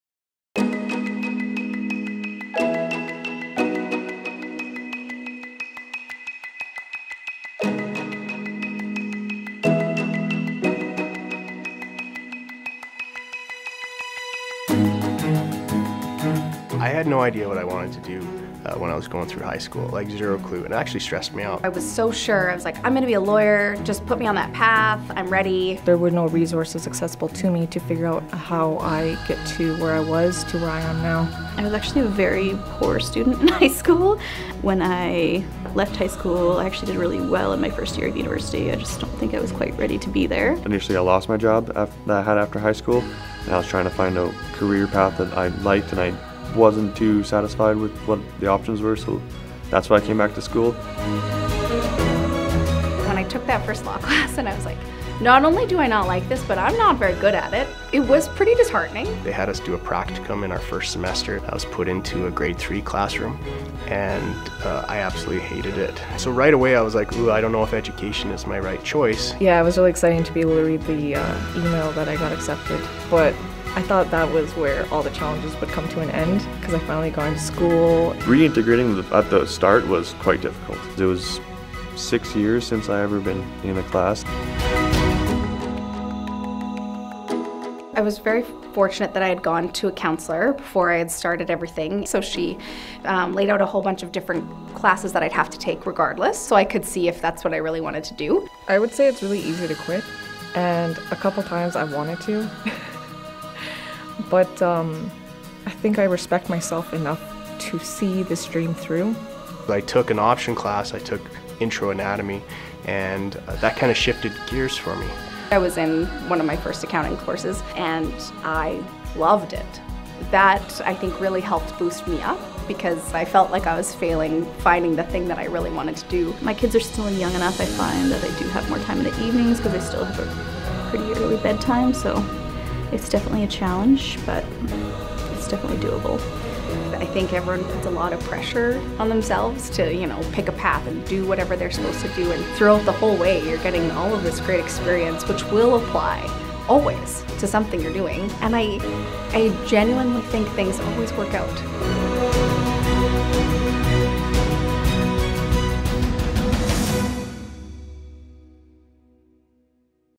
Your Path, Your Way | Medicine Hat College
Life’s journey is rarely a straight or simple path.
Not sure where you're going? You're not alone.

Hear from people who haven't been certain about what they want to do with their life. Who have been apprehensive about pursuing higher education. Who have needed support along the way.